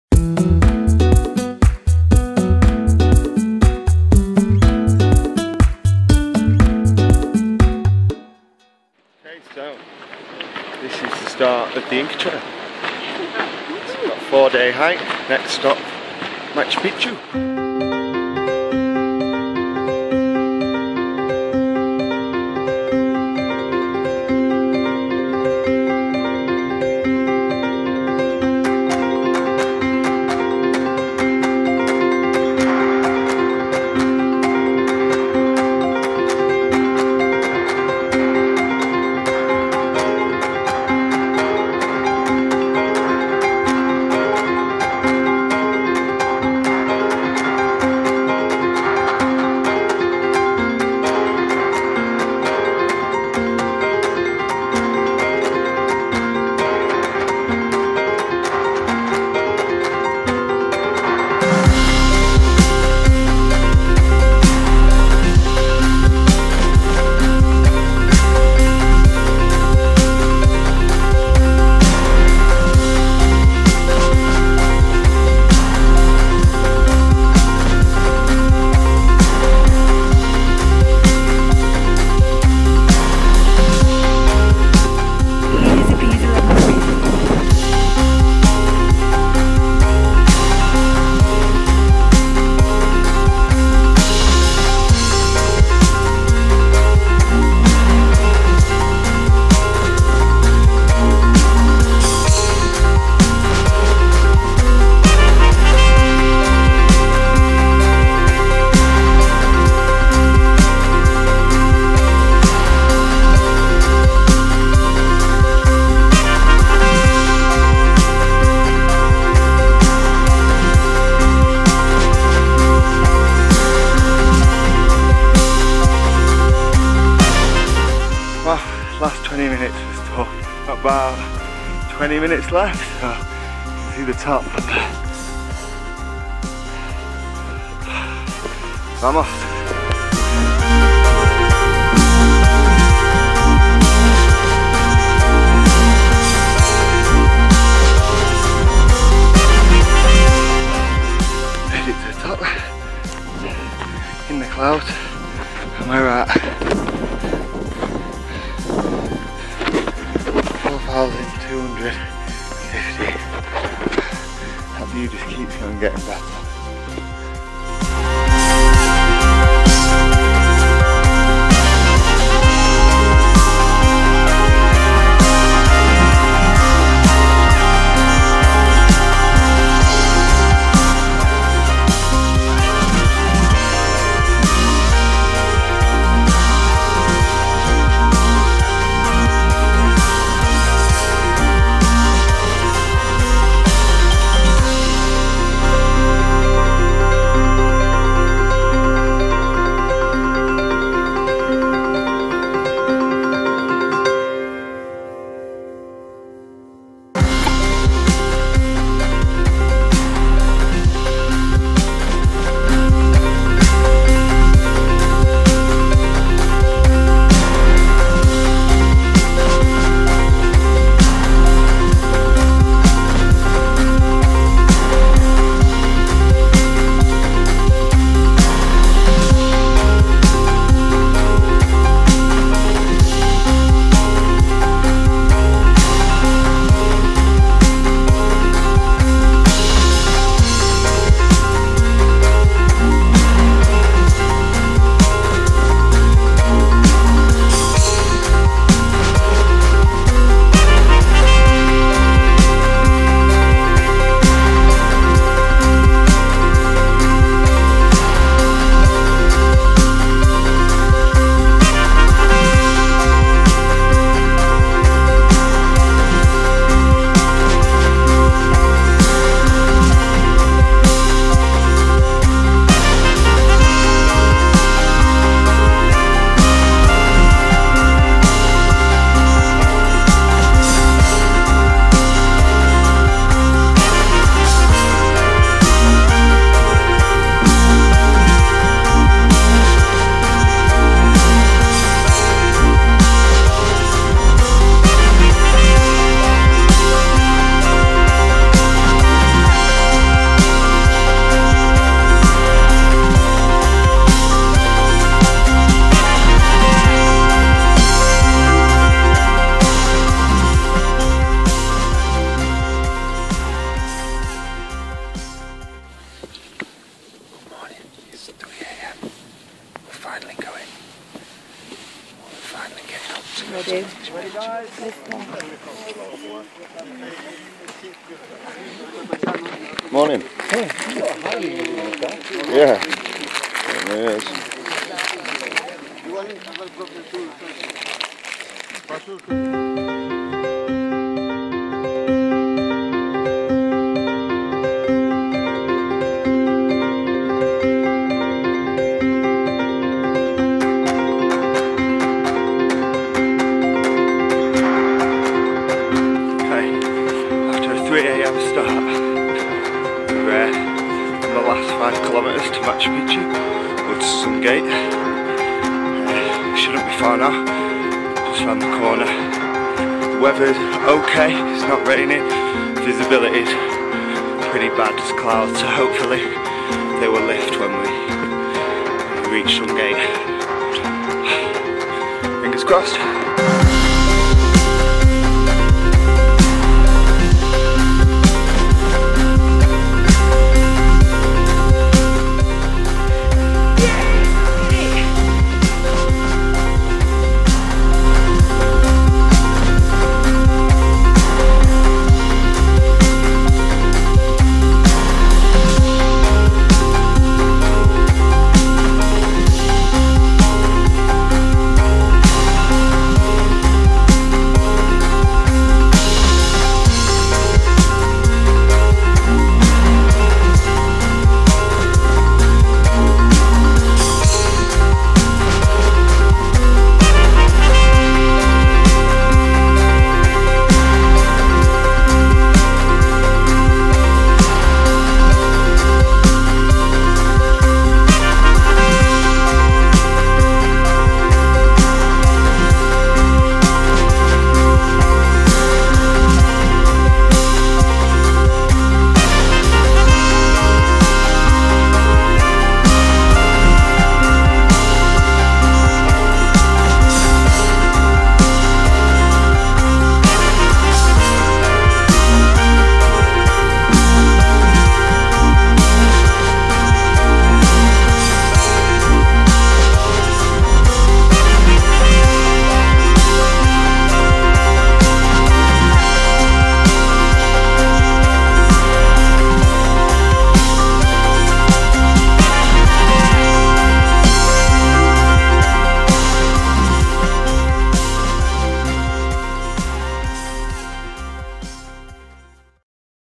Inca Trail with llama Path, Peru - Backpacking South America
Our 4 day Inca Trail Trek to Machu Picchu with Llama Path in Peru during our time travelling around South America